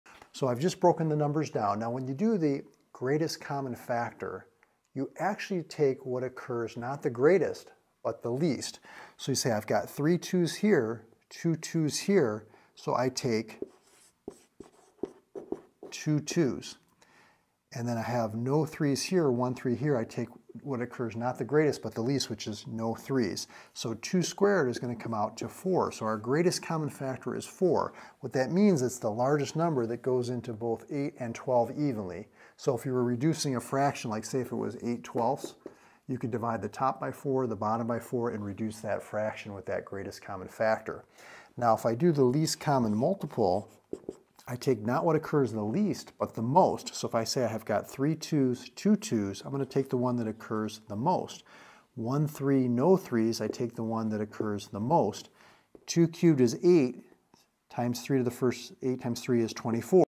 How to Find the GCF and LCM of 2 Numbers (Greatest Common Factor and Least Common Multiple)
In this short video we discuss how to find the greatest common factor and least common multiple of two numbers by using a factor tree.

Take Your Learning to the Next Level with Me (with the following options):

Learn Algebra 1 Video Course